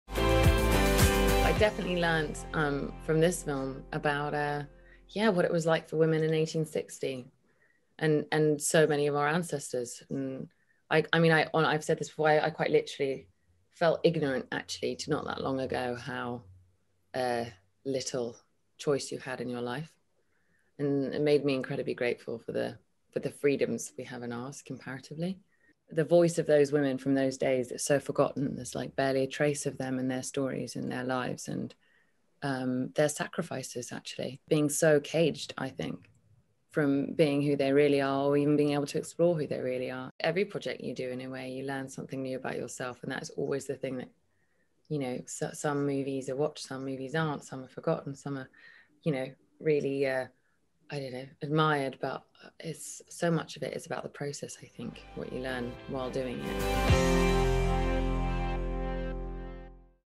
The World To Come | 2021 Sundance Film Festival | Adobe x IndieWire
At the 2021 Sundance Film Festival, we teamed up with IndieWire to talk to filmmakers about the importance of diversity in filmmaking and staying creative during times of adversity. Hear from the creators and cast of "The World To Come" - Mona Fastvold, Katherine Waterston, Vanessa Kirby, Casey Affleck, and Christopher Abbott.